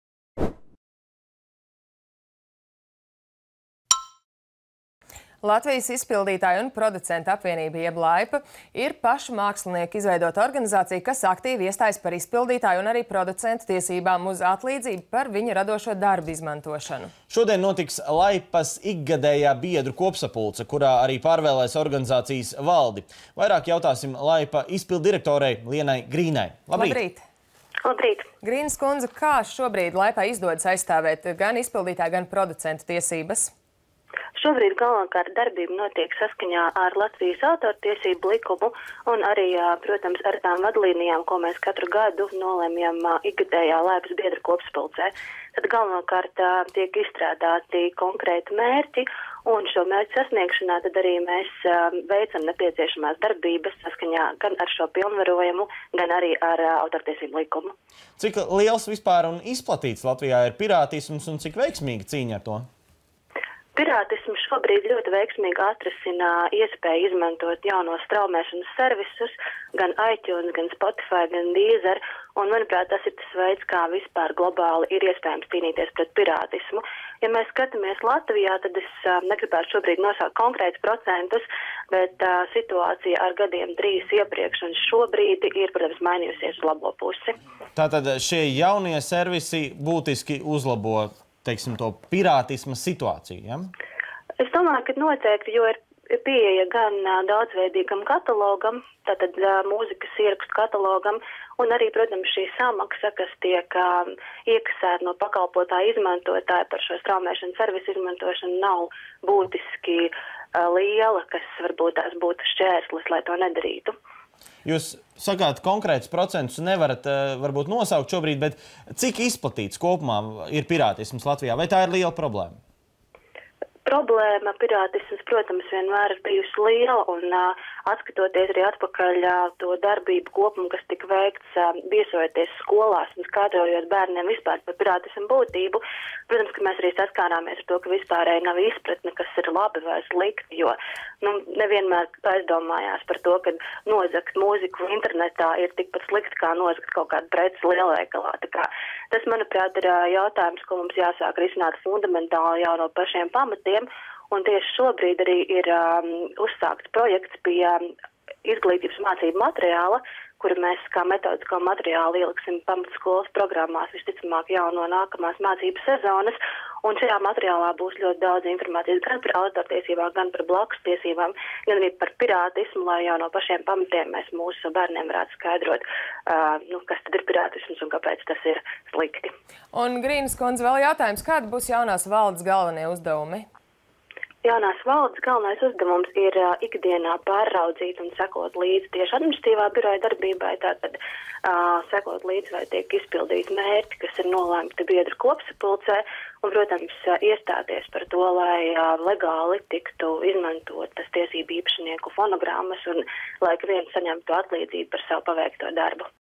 Telefonintervija ar LaIPA izpilddirektori Lienu Grīnu
“Latvijas izpildītāju un producentu apvienība” jeb LaIPA ir pašu mākslinieku izveidota organizācija, kas aktīvi iestājas par izpildītāju un producentu tiesībām uz atlīdzību par viņu radošo darbu izmantošanu.
Šodien notiks LAIPAs ikgadējā biedru kopsapulce, kurā arī pārvēlēs organizācijas valdi. Vairāk jautāsim LaIPA izpilddirektorei Lienai Grīnai.